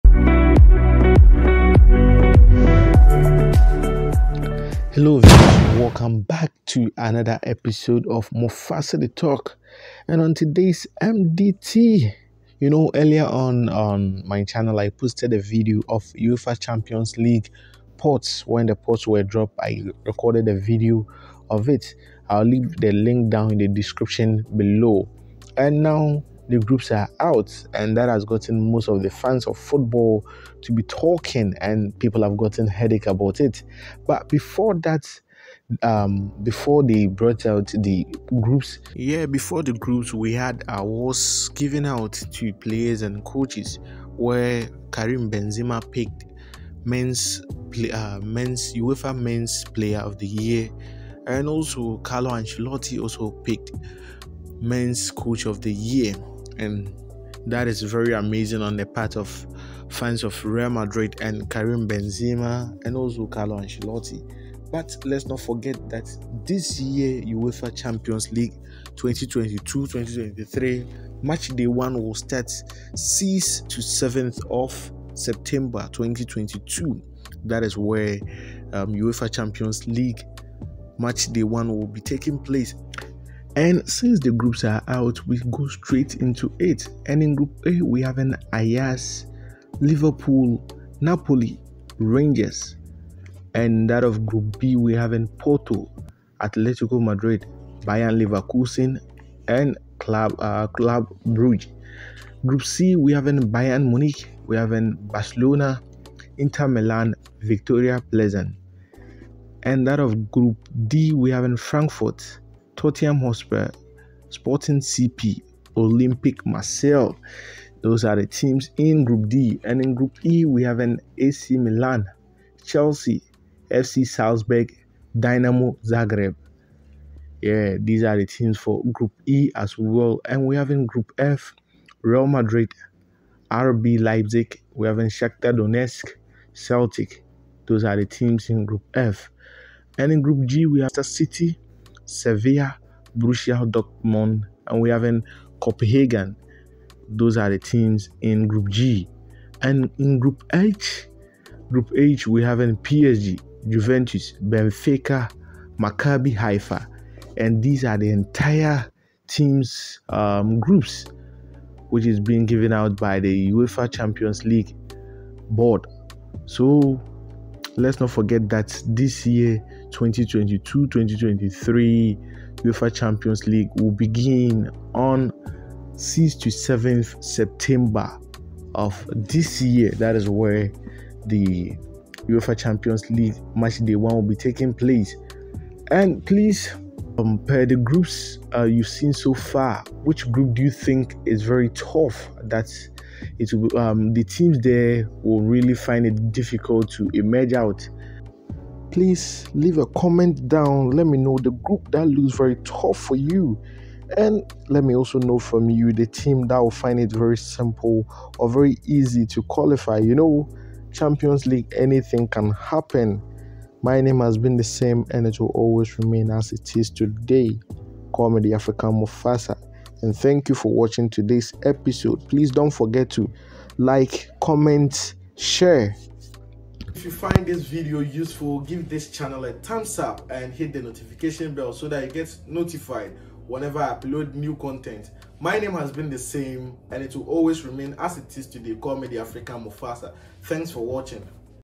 UEFA Champions league groups for 2022/23..
This video is about UEFA Champions league groups. In my previous video I recorded about the ports

This is how the UEFA Champions league groups from group A - H looks like...

Group A
Ajax
Liverpool
Napoli
Rangers

Group B
Porto
Atletico Madrid
Bayer Leverkusen
Club Brügge

Group C
Bayern Munich
Barcelona
Inter Milan
Viktoria Plzen

Group D
Frankfurt
Tottenham Hotspur
Sporting CP
Marseille

Group E
AC Milan
Chelsea
FC Salzburg
Dinamo Zagreb

Group F
Real Madrid
RB Leipzig
Shakhtar Donetsk
Celtic

Group G
Manchester city
Sevilla
Borussia Dortmund
Copenhagen

Group H
PSG
Juventus
Benfica
Maccabi Haifa

These are the entire groups for 2022/23 UEFA champions league